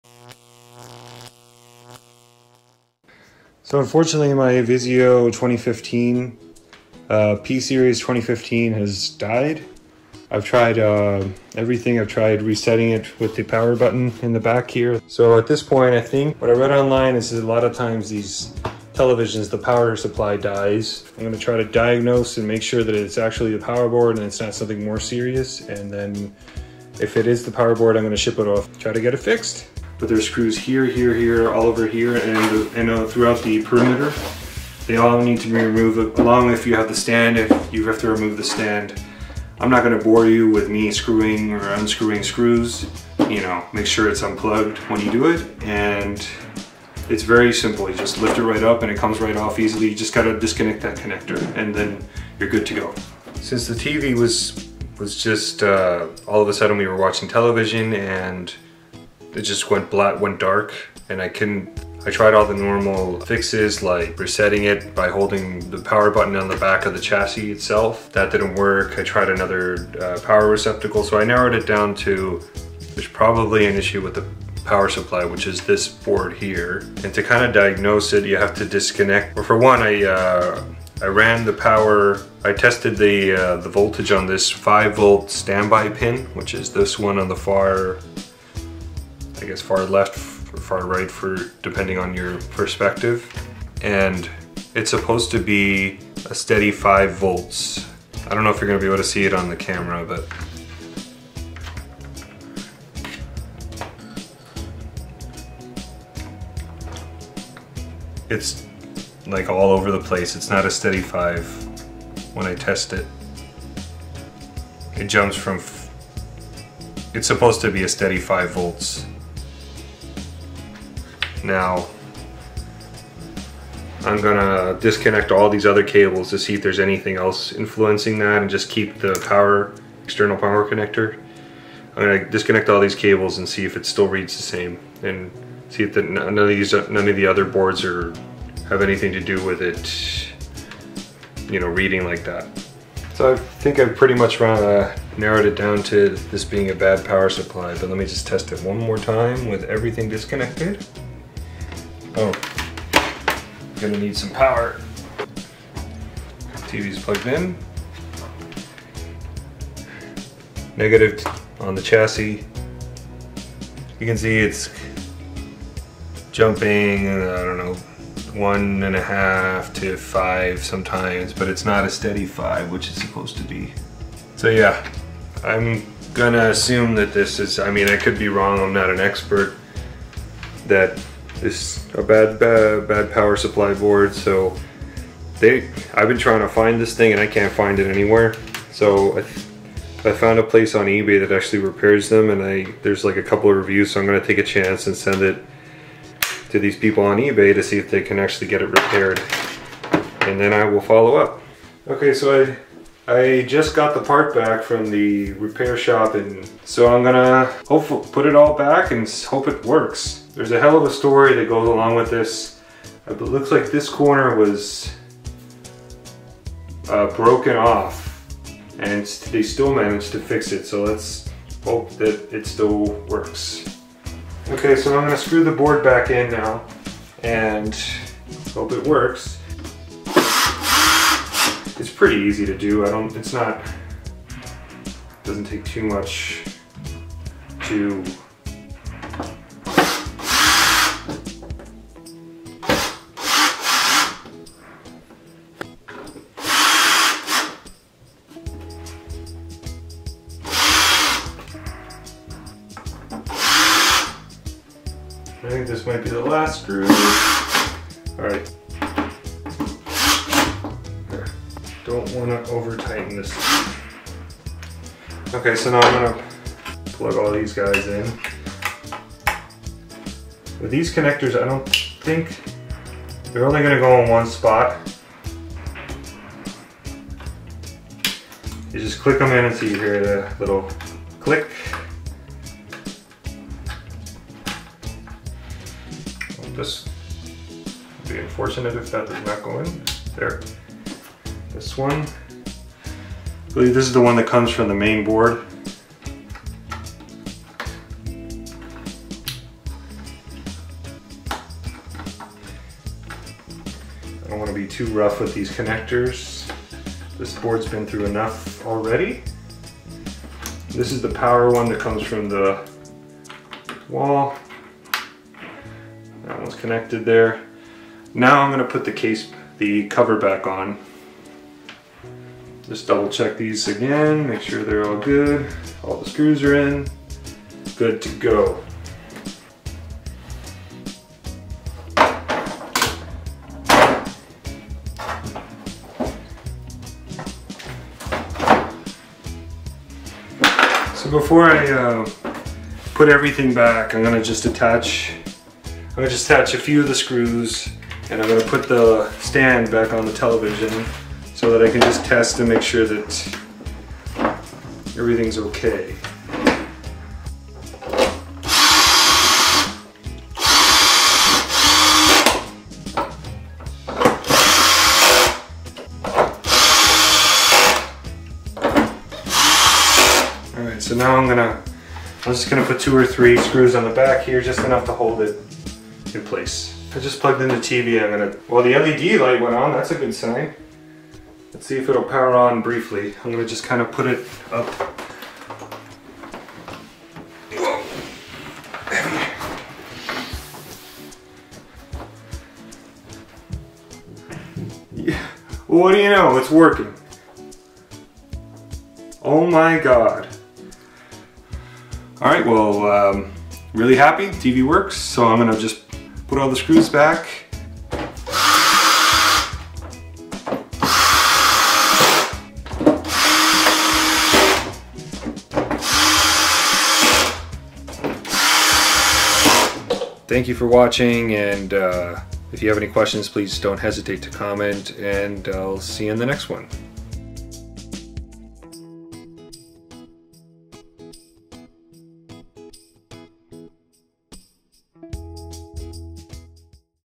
Vizio P Series Won’t Turn On? Power Supply Diagnostic and Repair
In this video I go over how I fixed my dead Vizio P Series 2015 (P652ui-B2).  The repair involved removing the TV's power board and sending it for repair.  If you have the same issue, I highly recommend the service I have listed below, which only cost $65.  They not only fixed the issue with power board but also managed to fix the damage that occurred during shipping.

Chapters:
00:00 Intro
0:30 Opening the TV
0:54 Diagnosis
3:21 Reinstalling the Power Supply Board
5:30 Reinstalling TV Cover
6:17 Testing the TV